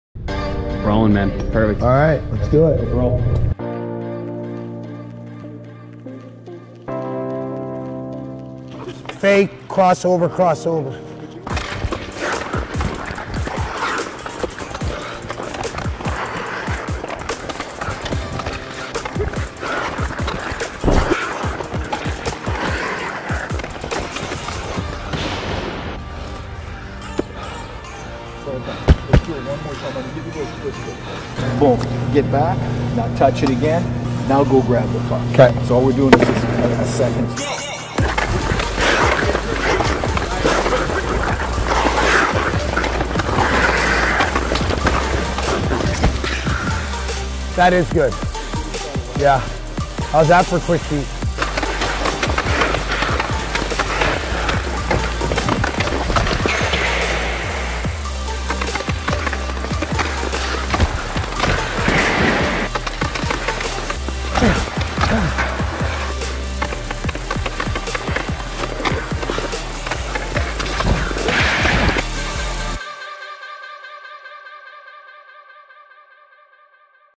MIC'D | P.K. GoPro Footage
We put a GoPro on P.K. Subban for his skate at RWJBarnabas Health Hockey House.